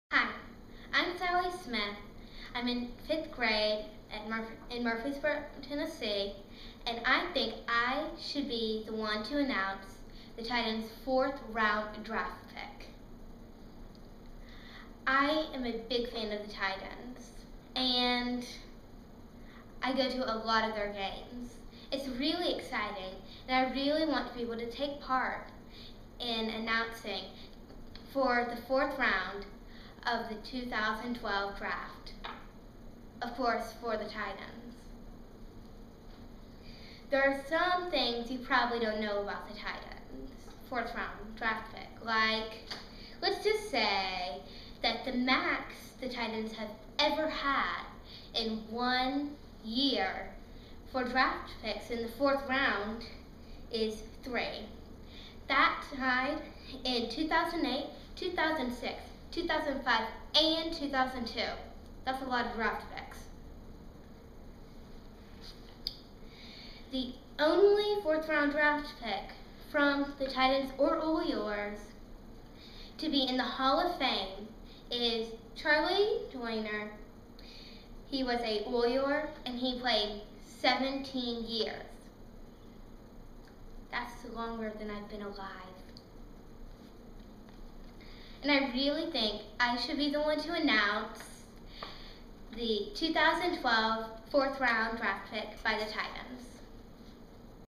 Sally Smith, age 11, auditions for the Tennessee Titans to announce their Fourth Round pick at the NFL Draft in New York City.